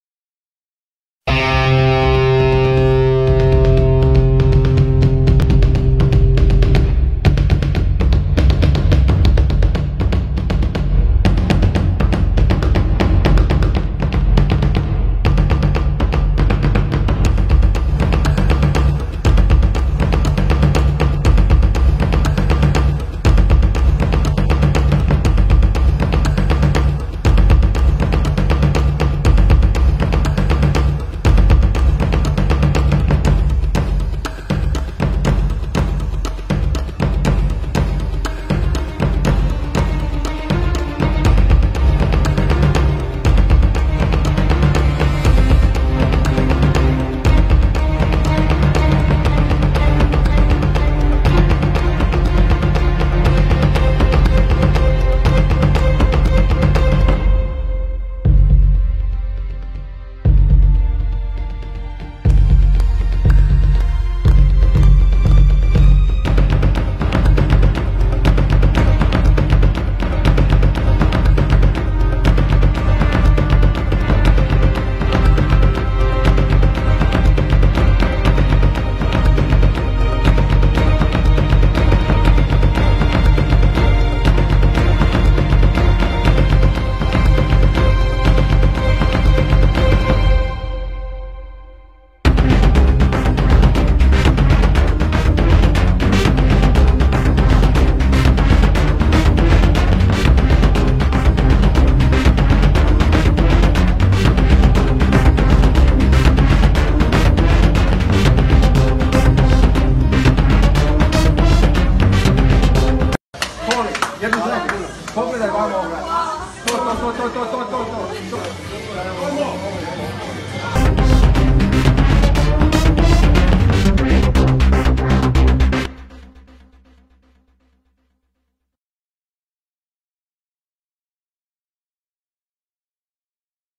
E bukura, emocionuesja dhe femra e hekurt, Kolinda Grabar-Kitarovic
"Ju mund t'i kuptoni njerëzit vetëm atëherë kur do ndiheni njësoj si ata", mbase kjo është shprehja që do ta përkufizonte presidenten kroate, 50-vjeçaren Kolinda Grabar-Kitarović.

Veprimi i saj empatik në finalen e Botërorit emocionoi jo pak njerëzit në mbarë botën. Por kush është Presidentja e Kroacisë, e njohja e këtij vendi në arenën botërore, nga femra e dashur e empatike, por edhe ajo femra e fortë që vuri pas hekurave "ish"-burra shteti...

Ndiqni këtë Speciale të përgatitur për presidenten e konsideruar si më seksi në gjithë botën...e cila vjen nesër, në Tiranë!

Realizoi: Edmond Prifti Multimedia

17 korrik 2018

Kolinda Grabar-Kitarović navijači
Croatia